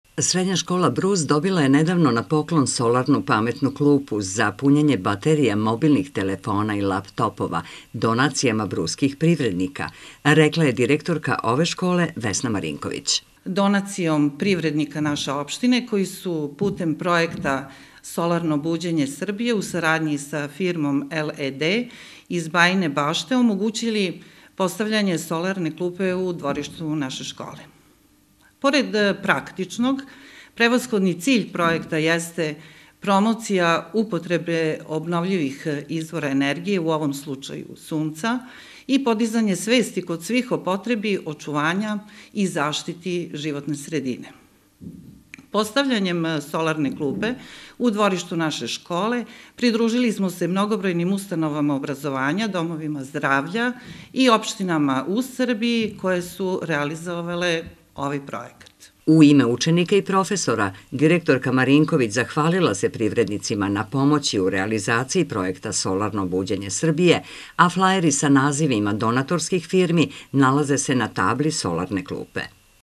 Solarna klupa u dvorištu Srednje škole Brus
Srednja škola Brus dobila je na poklon solarnu pametnu klupu za punjenje baterija mobilnih telefona i lap-topova. Reč je o donaciji bruskih privrednika, ističu u ovoj obrazovnoj ustanovi. U okviru projekta "Solarno buđenje Srbije" u saradnji sa firmom LED iz Bajine Bašte, solarna klupa je postavljena u školskom dvorištu. Cilj projekta jeste promocija upotrebe obnovljivih izvora energije, u ovom slučaju Sunca i podizanje svesti kod srednjoškolaca o potrebi očuvanja i zaštite životne sredine. U ime učenika i profesora direktorka škole Vesna Marinković, zahvalila se privrednicima na pomoći u realizaciji pomenutog projekta a flajeri sa nazivima donatorskih firmi nalaze se na tabli solarne klupe.